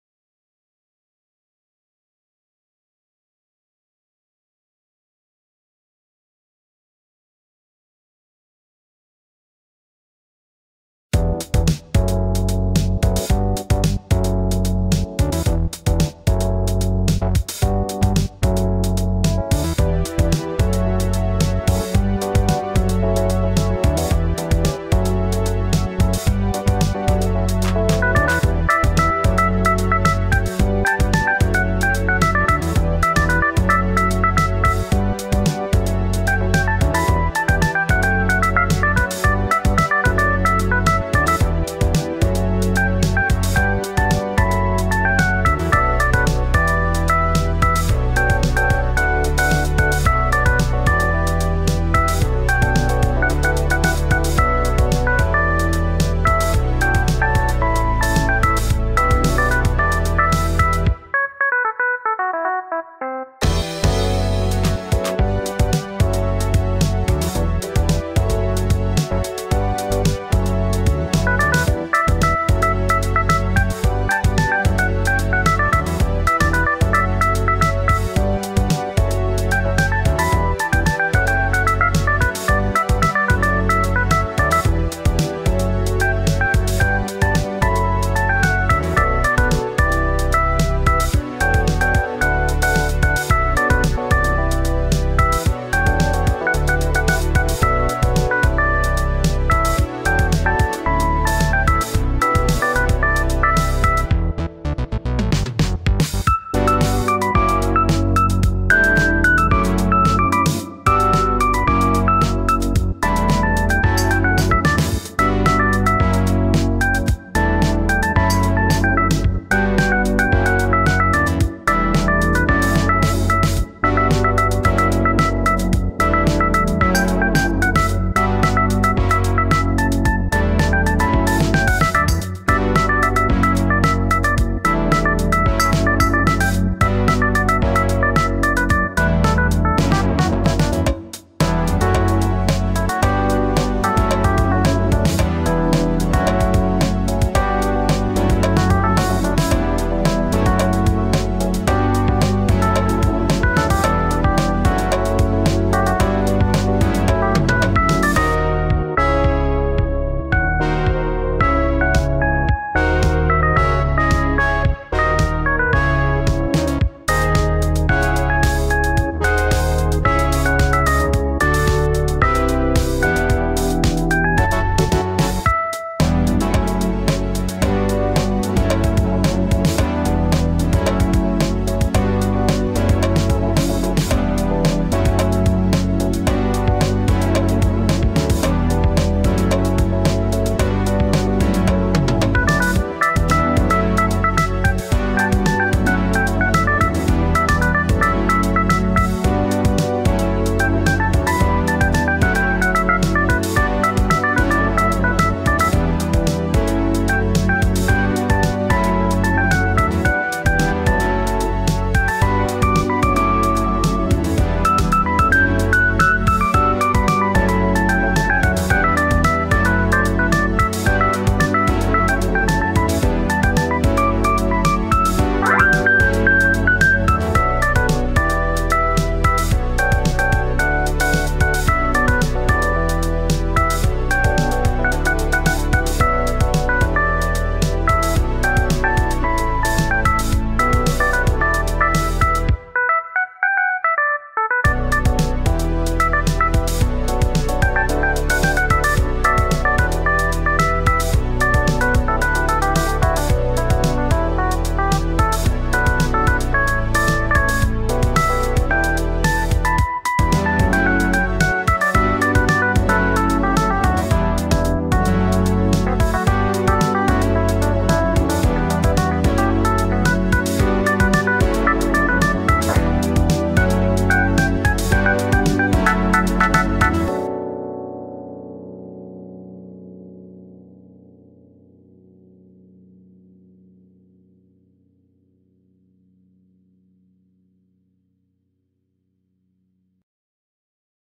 Jazz POP
Jazz-pop, what could be pop with some Jazz. The idea is explained a bit more in the intro to the video. If it were to actually be a pop song, I would imagine a melody different than that of the EP melody there is now.